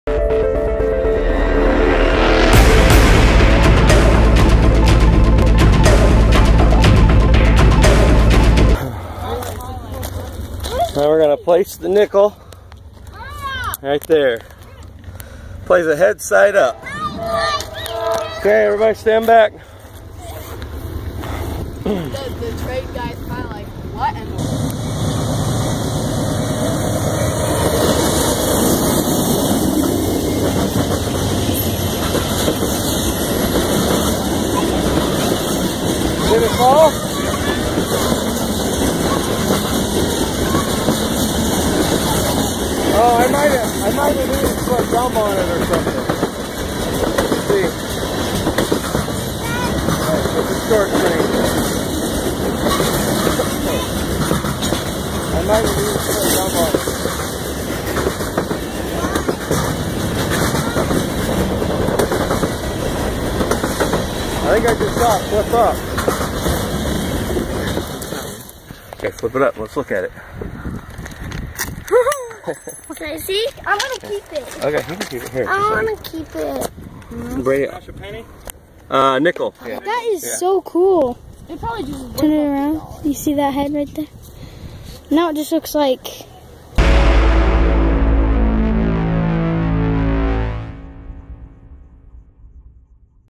train smashes a nickel
what to do when your bored but get a penny or a nickel, put it on the railroad track and watch the train smash it!
lazy summer days in the mighty midwest